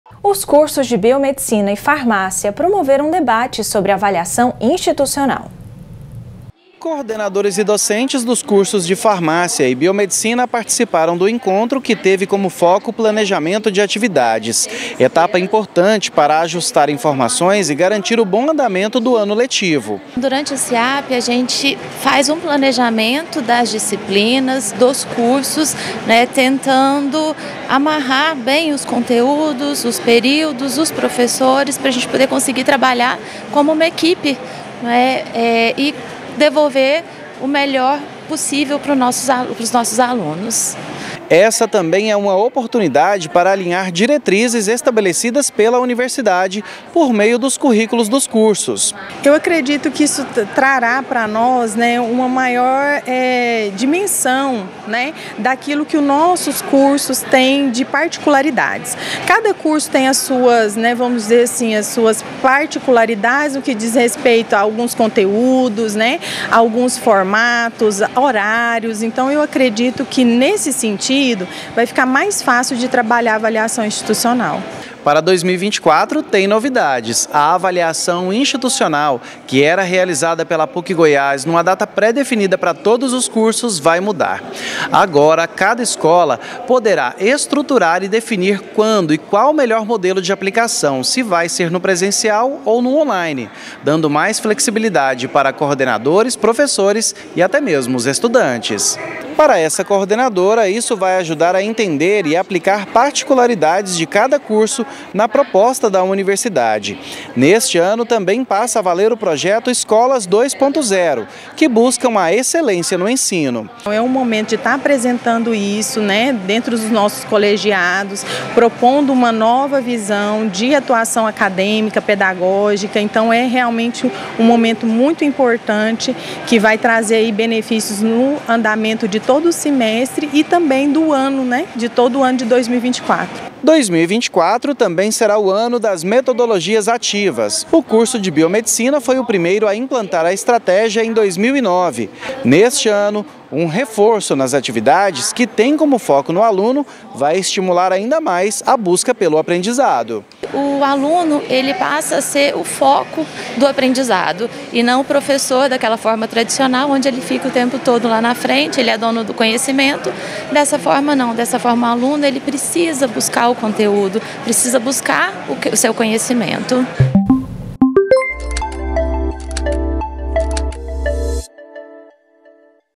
DOCENTES SE REÚNEM PARA DISCUTIR SOBRE O DESENVOLVIMENTO DA AVALIAÇÃO INSTITUCIONAL DA PUC GOIÁS
Durante a semana de integração acadêmica e planejamento, da PUC Goiás, docentes se reuniram para discutir o desenvolvimento da avaliação institucional, e o planejamento para este ano. Para 2024 existem algumas novidades, por exemplo: a avaliação institucional, que antes era realizada em uma data pré-definida pela universidade para todos os cursos, vai mudar; agora, cada escola poderá estruturar e definir quando será e qual o modelo de aplicação, sendo presencial ou on-line, dando maior flexibilidade aos coordenadores, professores e alunos. Confira!